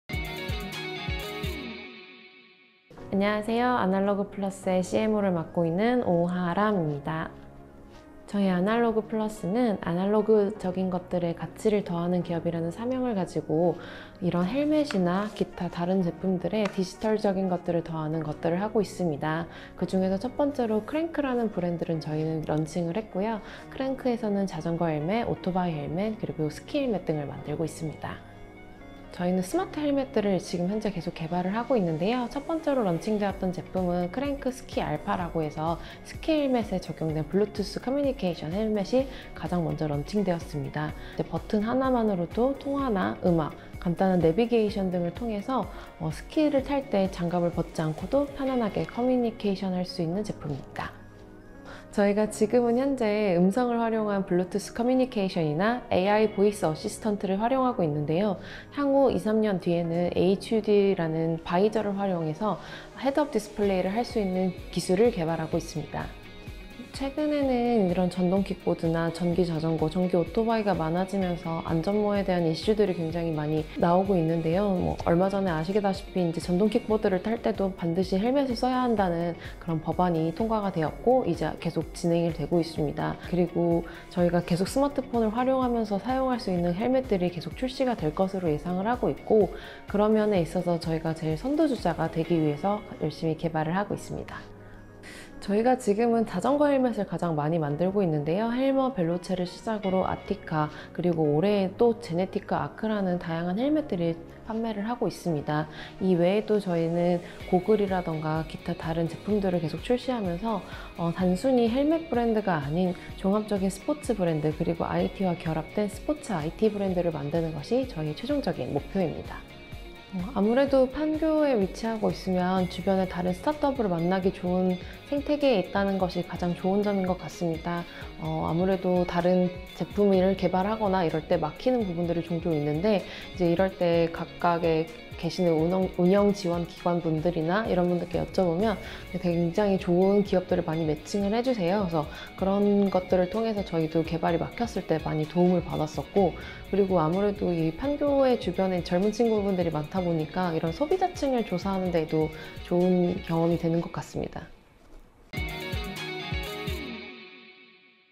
CRNK, a smart helmet brand that adds communication with IOT and eliminate the risk factor
As demand for individual movement devices and proxy courier services is soaring, not only consumer convenience but also safety concerns have risen. With the end of a month of guidance period, according to traffic laws revised on the 13th, it became mandatory for electric scooter riders to wear a helmet. It has ushered in an era where the users of personal mobility vehicles must wear a helmet.
In Pangyo Techno Valley Start-up campus, I sat down with CMO Haram O from Analogue Plus to listen to an explanation of “CRNK,” which combined IoT with a safety helmet.

Q1. Company introduction
CMO Haram O: With its mission of adding value to everything analogue, Analogue Plus is adding digital elements to helmets among other products. Now, through a brand specialized in sports using IoT “CRNK,” it is producing and selling communications devices, bicycles, motorcycles, ski helmets and sun protection umbrellas. It has started as an internal startup at Samsung electronics and set its first aim to manufacture smart helmets.

Q2. What is the current status of helmet development combined with IoT?
CMO Haram O: The development of smart helmets is currently underway. The first product launched was CRNK’s “Ski Alpha.” Bluetooth communications system is applied to these helmets. With the click of a button, users can listen to music, make a phone call or choose navigation. Also, users do not have to take off their gloves to communicate since it is a touch-tone device. We started to produce a smart helmet because it not only helps users secure a clear view but also allows them to listen and speak freely. Now, a helmet attached communications device “Ahead M” is also for sale.
Q3. Future goals
CMO Haram O: Today, we are using bluetooth communications using voice or AI voice assistants. We are making efforts to create a head-up display through HUD visor in the coming two to three years. It is projected that helmets where smartphones can be utilized will be released going forward. As a leader in the industry, we are working hard. Now, the majority of production goes to bicycle helmets. There are various types of helmets from HELMER, VELOCE, ARTICA to GENETIC and ARC which are released this year. Our ultimate goal is to create a multi-sport brand using the IT technology which is simply not a typical helmet brand while releasing other sports products.
Q4. Why did you choose Pangyo?
Q5. Goals moving forward
CMO Haram O: Being located in Pangyo, the biggest benefit is to interact with other start-ups. There have been several moments when we hit a wall in the development stage. At the time, an operation support agency has matched us with excellent companies. We have received a lot of help back then. Since there are many young people around the Pangyo area, it helps when conducting a survey on consumer groups.